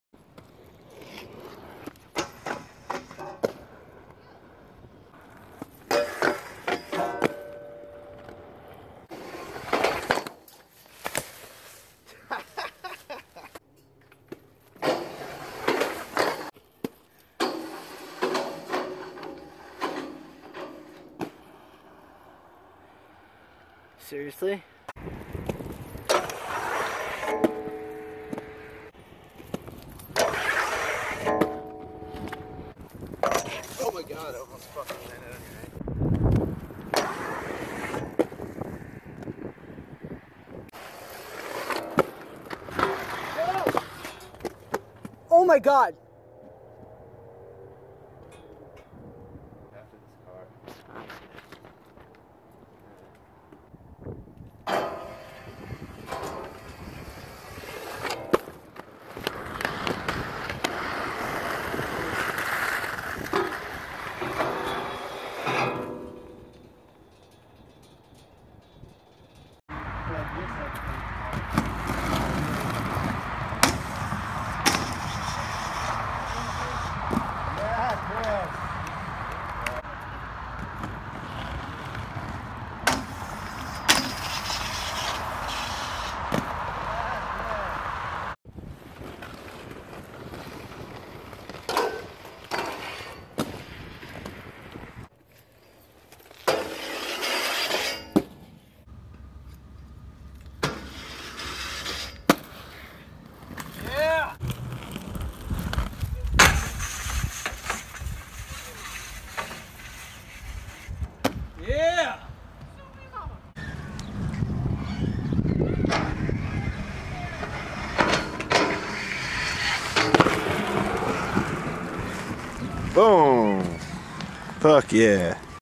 FA℞M x USD AEON SIXTY
April 22, 2018 - May19, 2018
Minneapolis/St. Paul, Minnesota

Been Aeon these skates for just over a month now...  Here's a compilation of some clips I've acquired chronologically, except for the last clip as it was the actually the first...

               Dave Walsh @davidalanwalsh